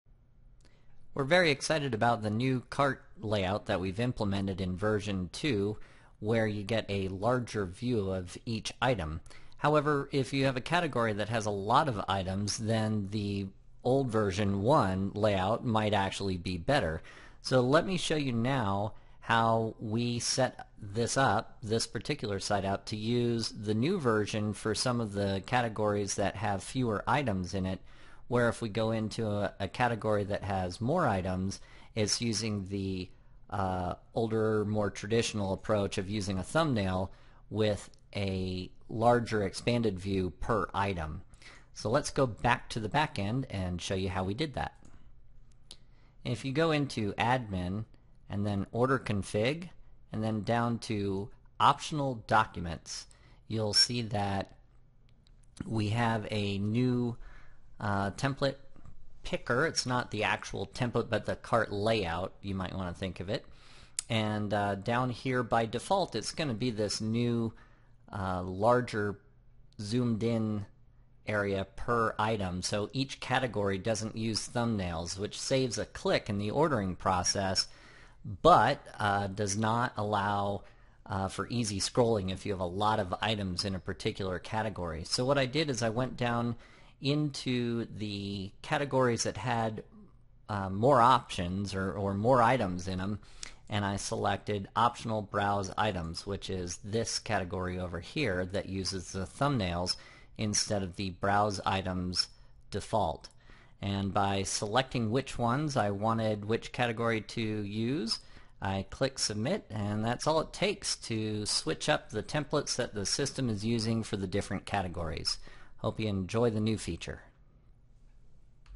Changing Cart Layout in ERS - Event Rental Systems
Here's how to change individual categories or your entire ordering system using different cart layouts.  You can choose from a thumbnail view for categories that generally have more items or the new default view that eliminates the thumbnails and directly shows the item view levels within each category.  A powerful new feature included in ERS.